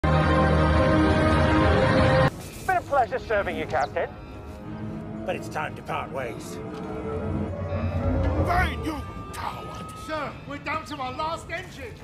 Vane the Pirate CONFIRMED in Skeleton Crew Star Wars
Vane the Pirate from the Mandalorian will be in the new Skeleton crew Star Wars show!

More clips like this on the channel!